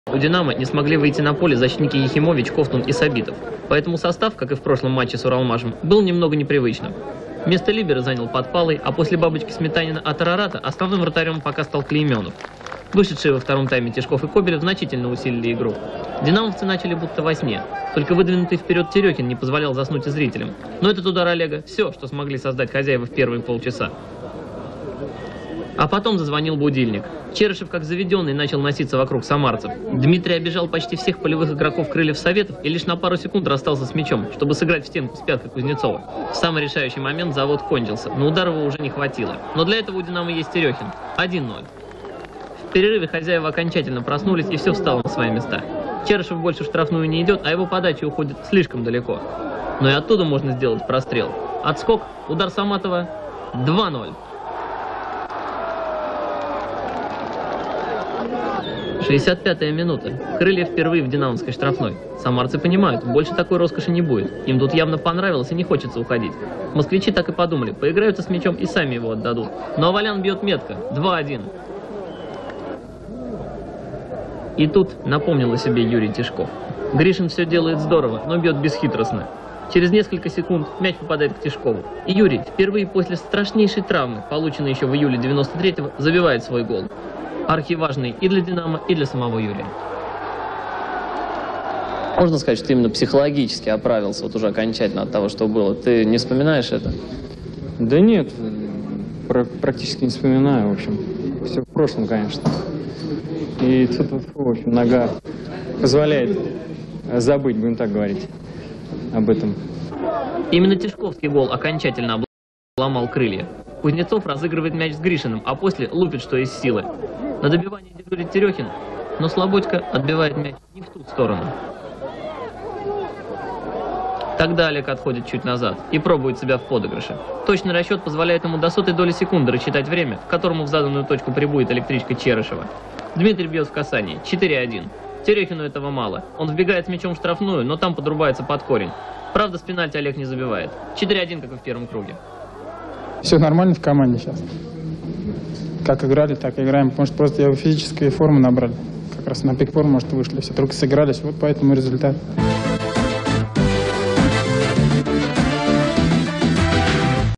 Динамо (Москва) 4-1 Крылья Советов. Чемпионат России 1995
Динамо (Москва) 4-1 Крылья Советов (Самара)
Чемпионат России 1995. 26-й тур.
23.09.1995
Москва, стадион "Динамо"